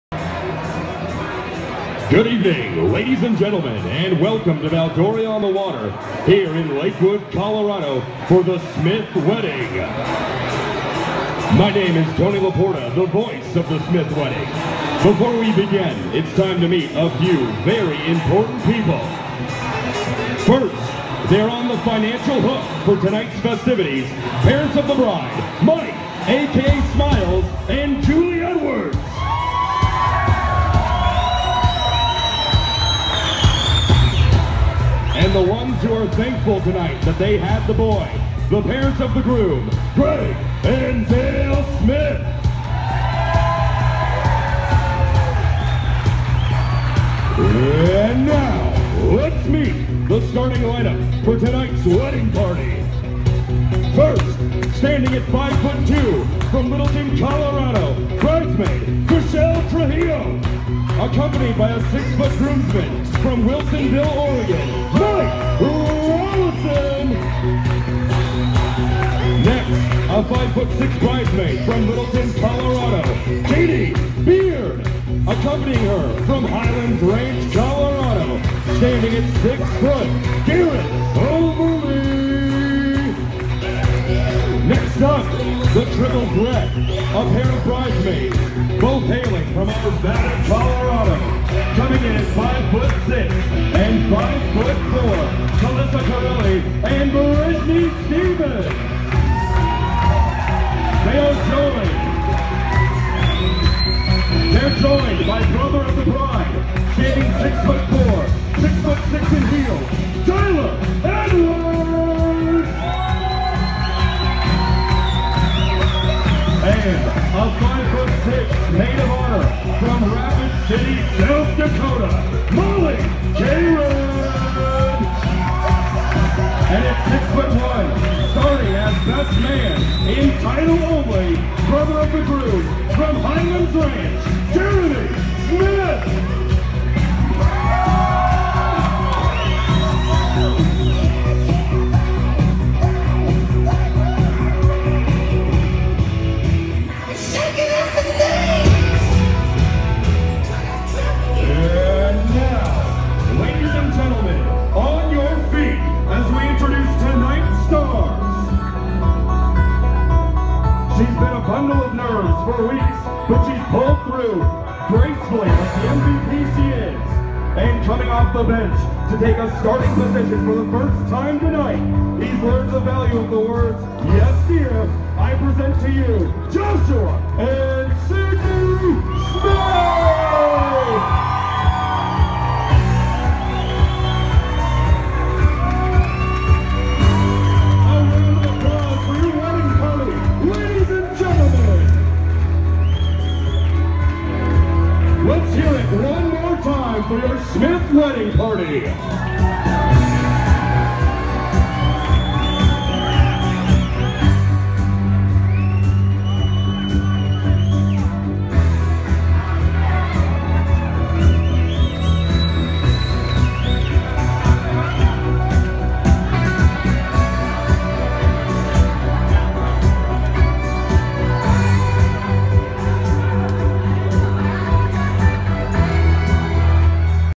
Introducing Smith Wedding party
Tony LaPorta introducing the starting line up for Josh and Sydney Smith's wedding NBA pre game style.,